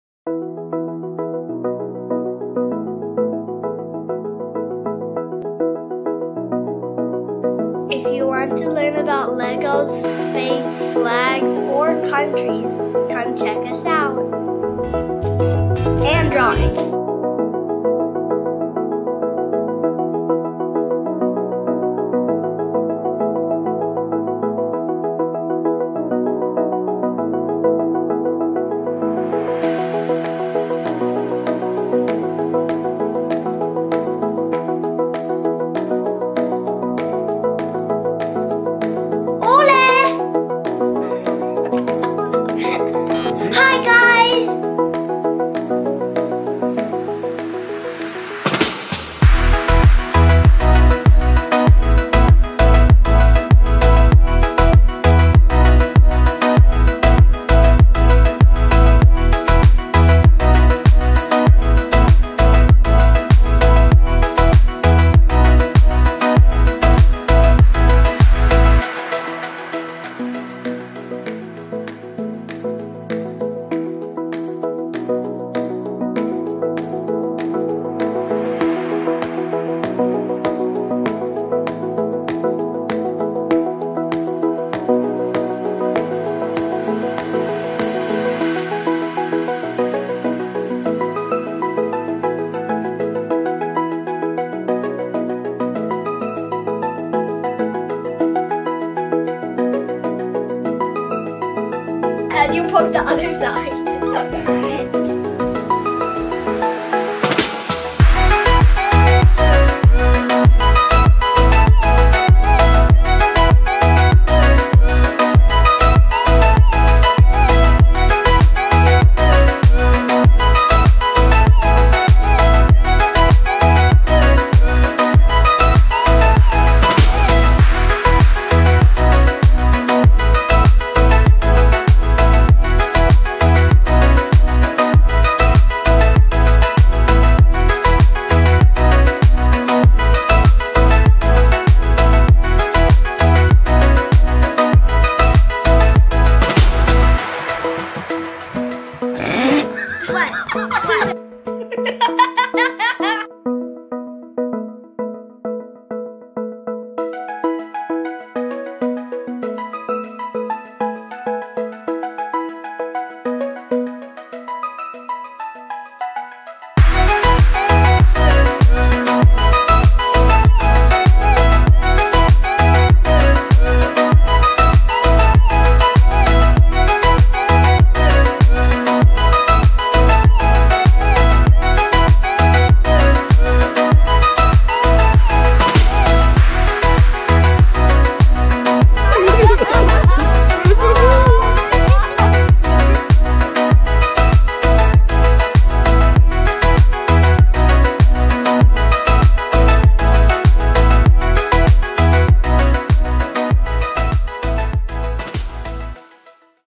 Intro - Short Videos of 2 Brothers from Long Island, N.Y.
Celebrating 40 short videos with an Introduction to Khizar and Eesa's channel! Enjoy!